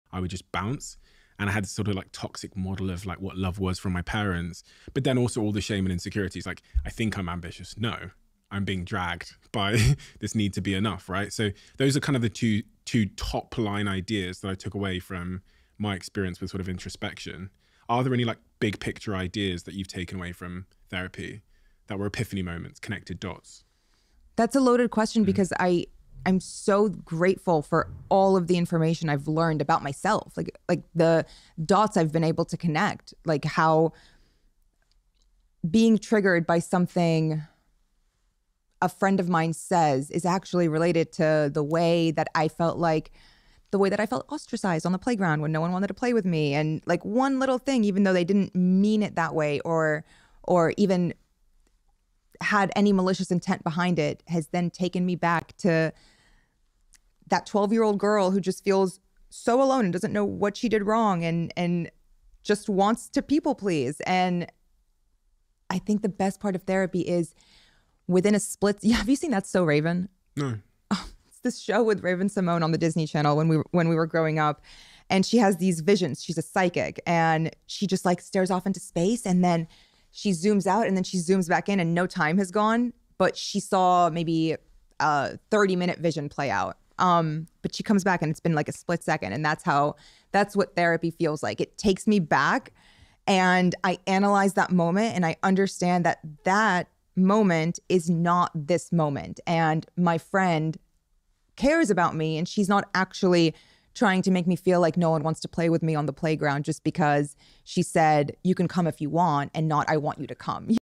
Understand the Power of Therapy: Epiphanies and Growth
Discover the transformative power of therapy as I share my personal experiences and insights. Learn how therapy helped me connect the dots, heal childhood wounds, and overcome insecurities. Join me on this journey of self-discovery and growth.